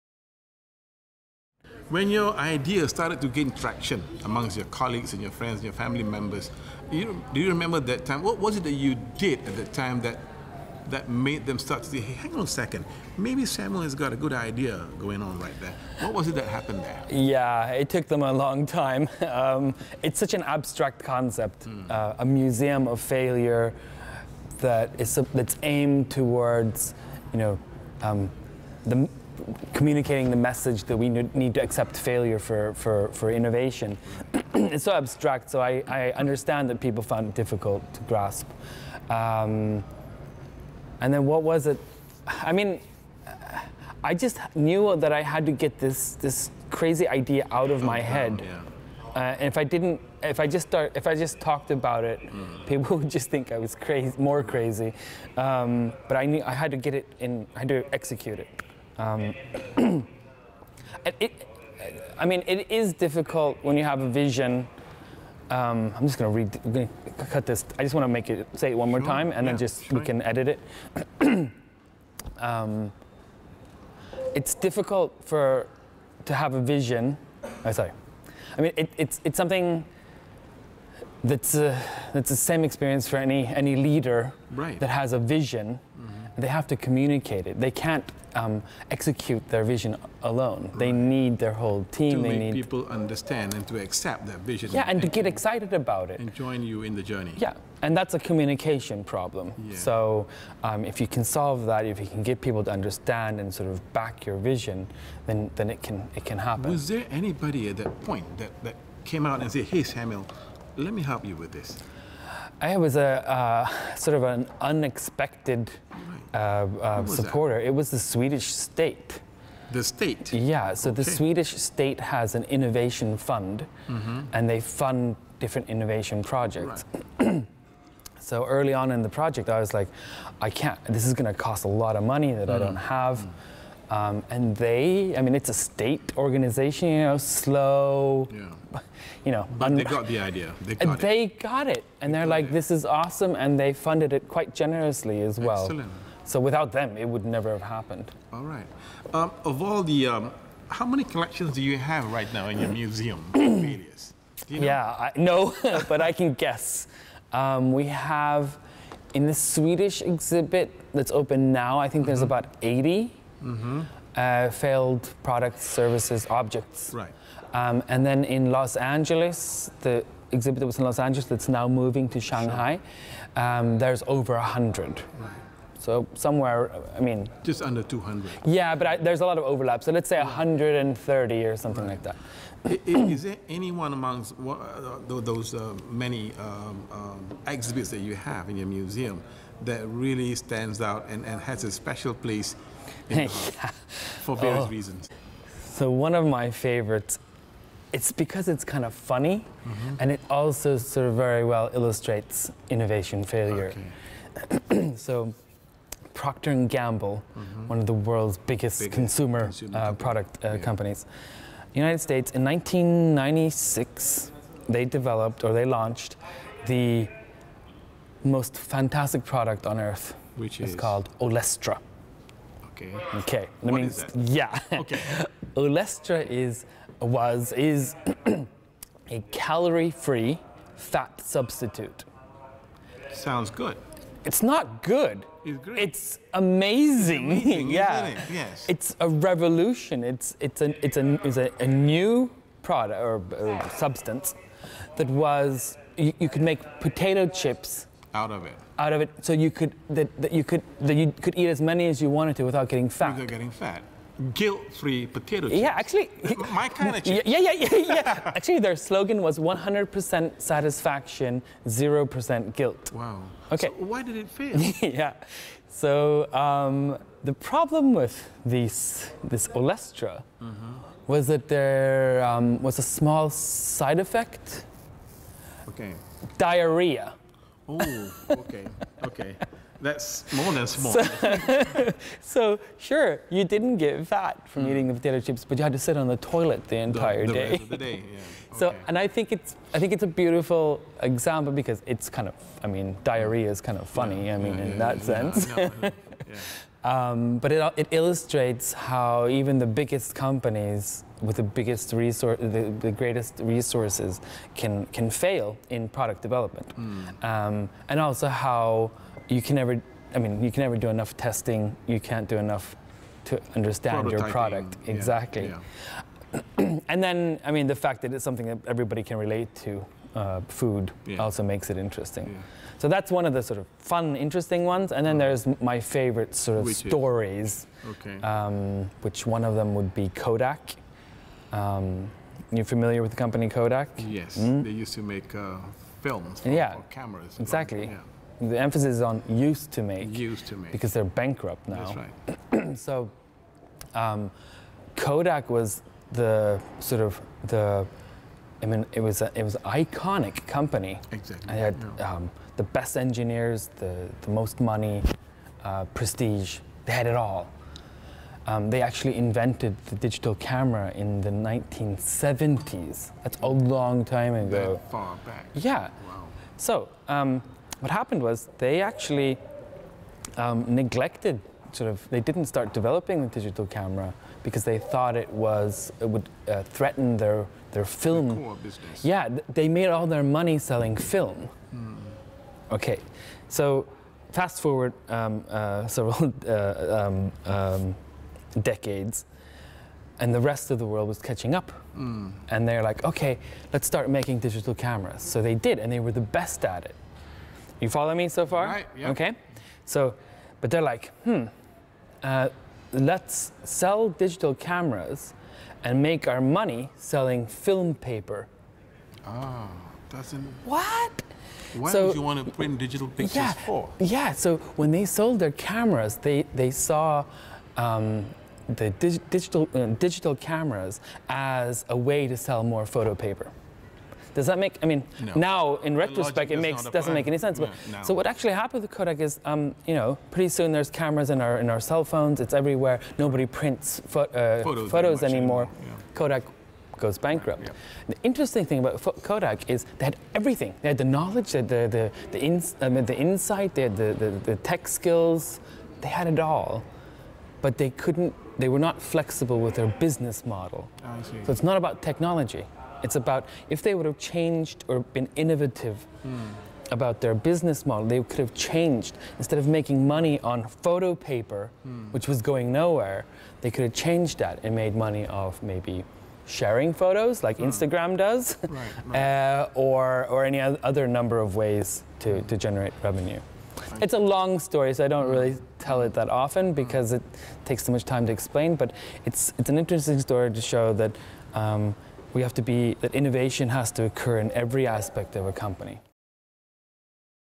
Interview with Dr. Samuel West | Vision Casting
In this second part of a fascinating interview series, Dr. West reveals several interesting principles.

How did the Museum of Failure gain traction? How did it become widely accepted? Dr. West discusses the corporate zeitgeist that failure needs to be accepted for innovation to happen. He also talks about the value of taking action: Instead of just talking about his idea, he went out to do it and then reaped the rewards.

Further, leaders need other people to execute a big vision. Thus, communication is key in rallying people to your cause.

The Museum of Failure also received funding from an unlikely source – the Swedish government. If it weren’t for these grants, the Museum of Failure would not be a reality. Thanks to their helping hand, the Museum now has 80 exhibits for the Swedish branch and 100 for the LA branch.

His favorite exhibit in the Museum of Failure? Proctor and Gamble’s Tostinos Chips, containing the fat-free and calorie-free substance Olestra. While a calorie-free, fat-free potato chip sounds promising on paper, the main issue was the unfortunate side effect of diarrhea.  This proves to Dr. West that even the biggest companies can fail in the process of product development.

Dr. West also talked about Kodak. Because of a lack of flexibility in their business model, they ended up bankrupt. Innovation therefore needs to occur in every aspect of the business, and not just in product development.

Dr. Samuel West is the curator for the Museum of Failure. Dr West is a licensed psychologist, PhD in Organisational Psychology. He’s also one of the funniest speakers at LESA 2018.